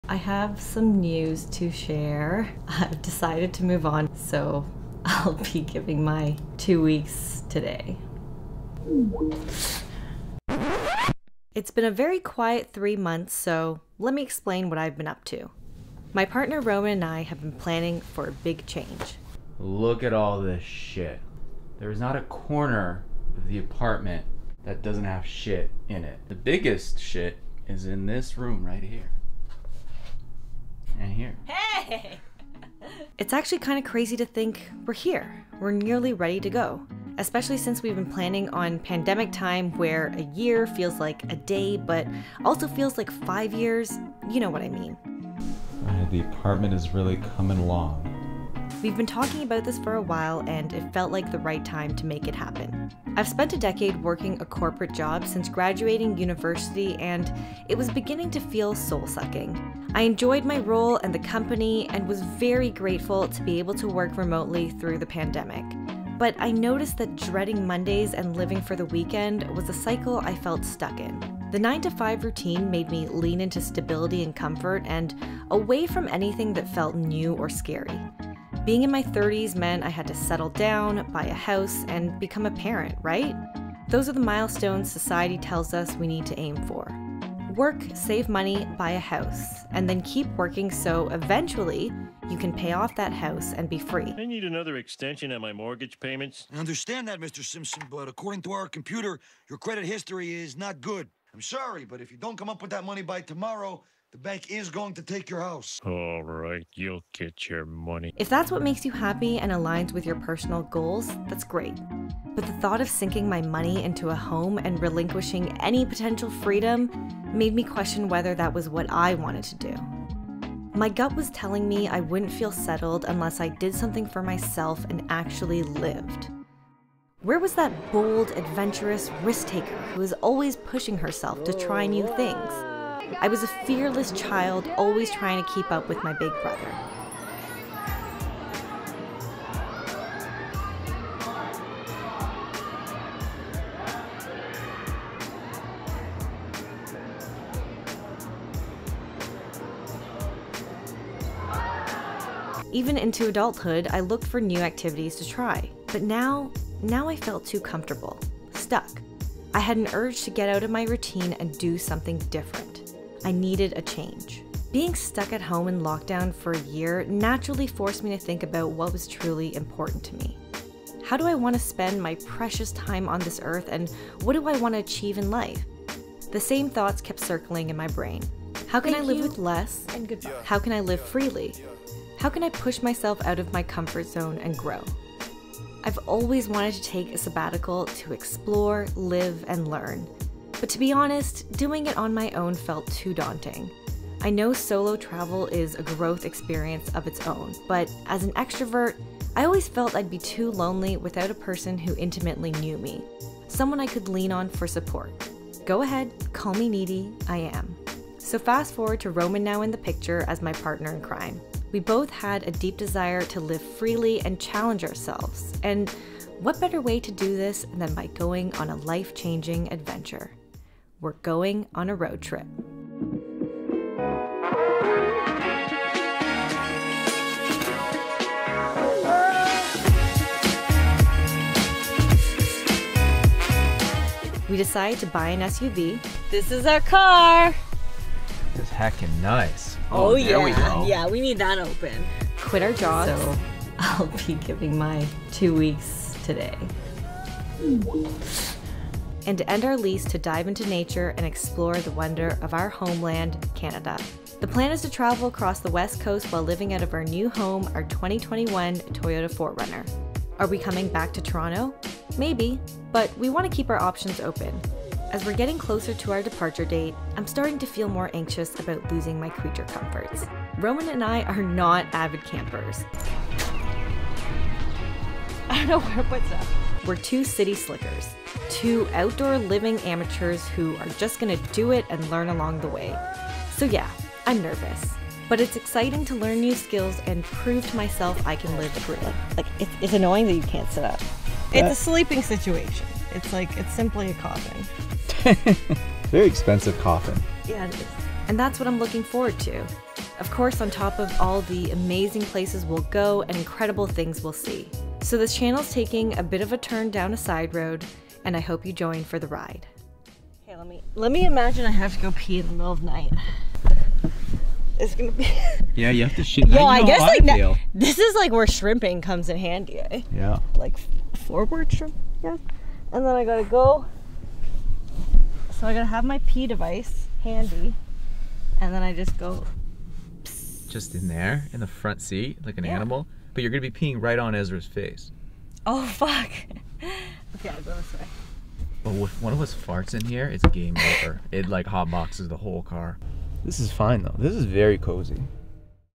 I quit my corporate job to go on an adventure
It's been a very quiet last few months so I wanted to fill you all in on what I've been up to...
I'm going on an adventure! 🚐

Sony A7S III
Tamron 20-40mm Lens
Rode VideoMic NTG
Manfrotto Tripod